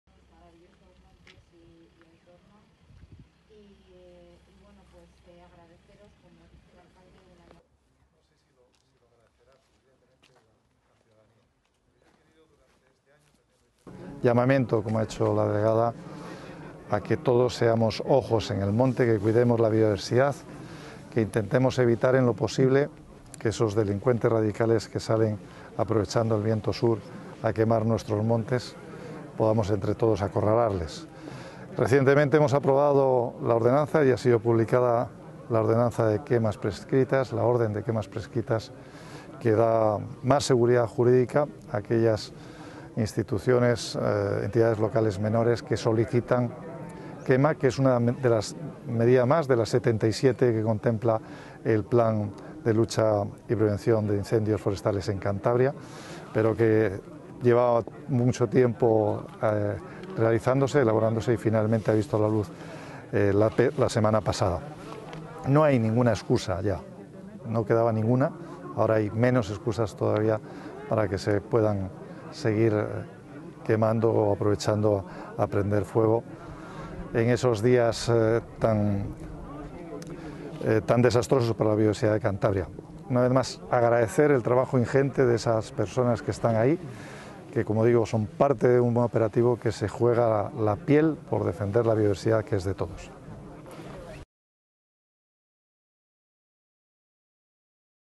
La BRIF vuelve a Cantabria
El consejero de Desarrollo Rural, Ganadería, Pesca, Alimentación y Medio Ambiente, Guillermo Blanco, ha apostado hoy por intensificar las medidas de prevención en la lucha contra los incendios forestales y reforzar la colaboración ciudadana y entre administraciones para “perseguir entre todos a los incendiarios que queman nuestros montes”.